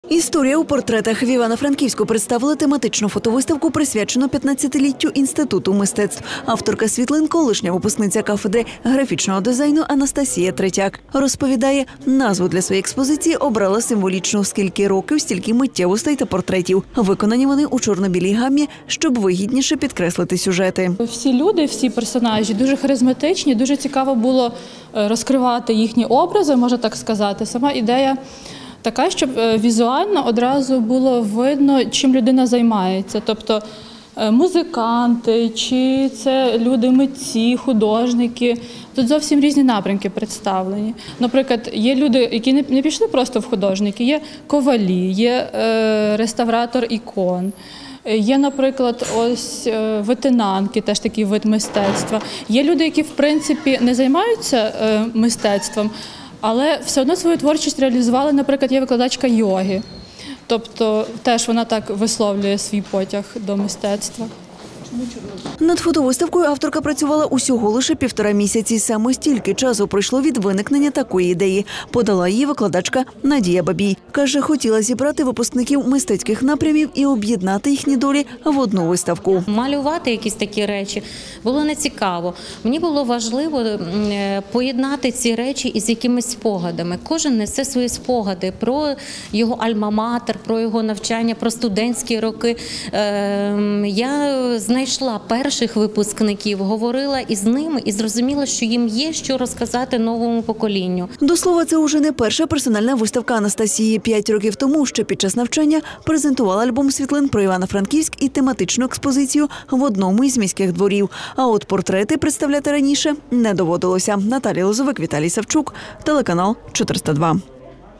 Фотовиставку до 15-ліття Інституту мистецтв представила Анастасія Третяк
Історія у портретах. В Івано-Франківську представили тематичну фотовиставку присвячену 15-літтю Інституту мистецтв.